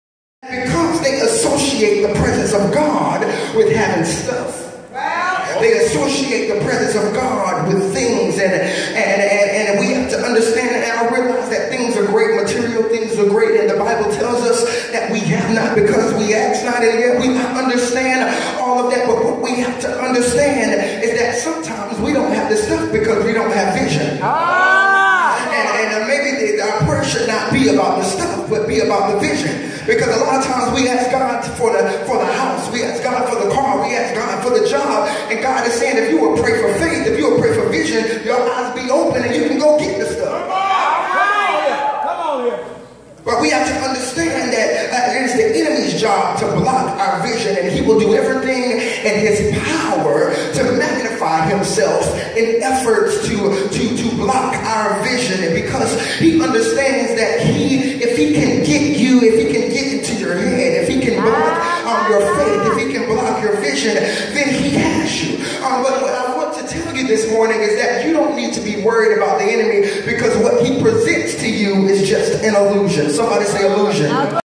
Pastor Reginald Boney
It's Time to Upgrade Your Vision The Rock Youth Church Sunday
Isaiah 43:19
vision stuff Do you have a vision? Pray for vision, pray for faith and go get the stuff